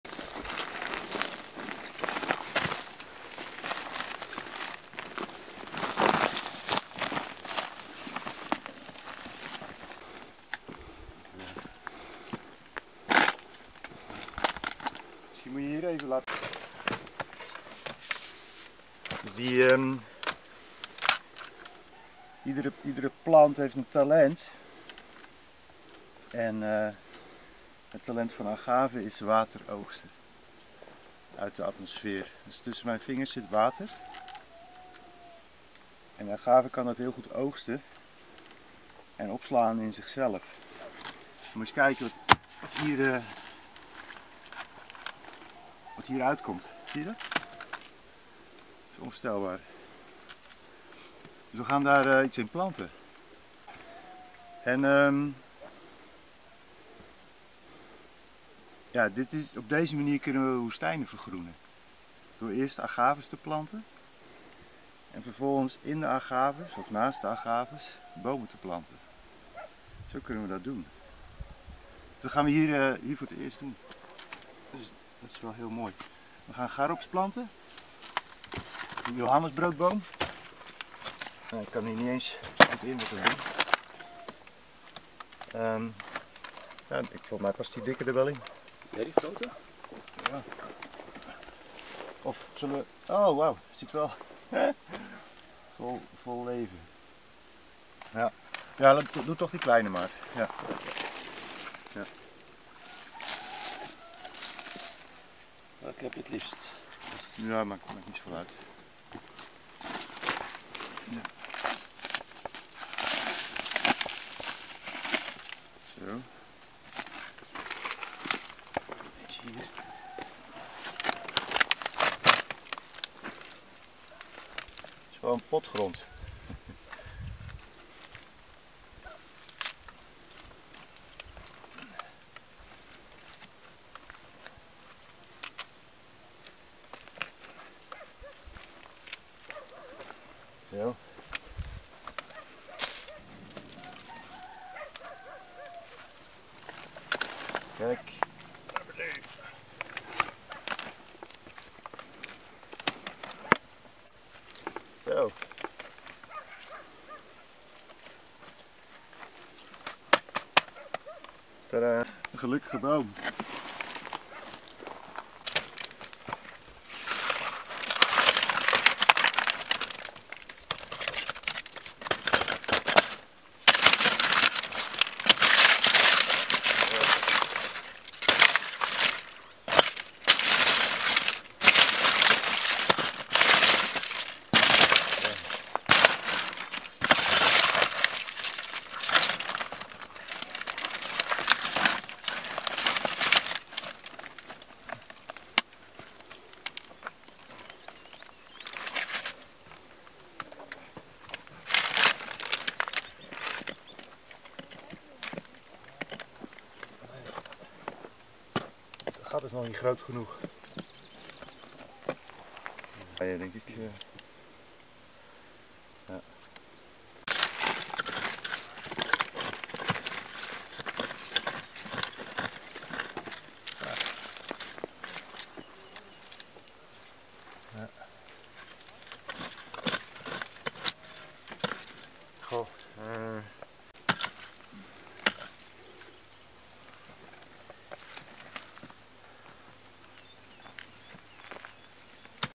متطوع هولندي يغرس أشجار الخروب في جذع الصبار الأمريكي الغني المياه... ما بعد الغرسما رأيكم؟
المتطوع الهولندي "يوس" في الضيعة الإيكولوجية إجرمواس (اقليم الدريوش)، يغرس أشجار الخروب في جذع الصبار الأمريكي الغني بالمياه والمواد العضوية المتراكمة
طريقة مبتكرة لمنح الغرس المياه والمواد العضوية الكافية في مرحلة ما بعد الغرس
ما رأيكم؟